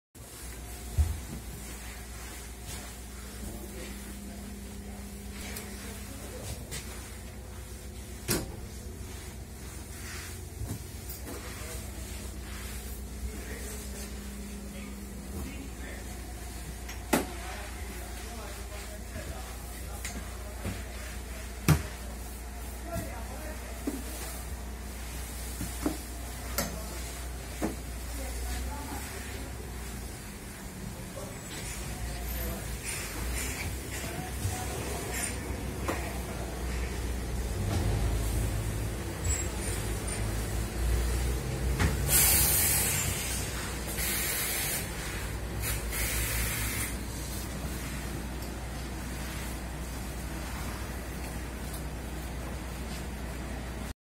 TUA - AL LAVORO PER LA SANIFICAZIONE DEI BUS
L'azienda del trasporto pubblico locale abruzzese, TUA, con sede centrale a Pescara, da circa una decina di giorni ha cominciato un intenso lavoro di disinfezione e sanificazione dell'intera flotta di bus. La prima risposta all'emergenza COVID-19, che tanto preoccupa il nostro paese.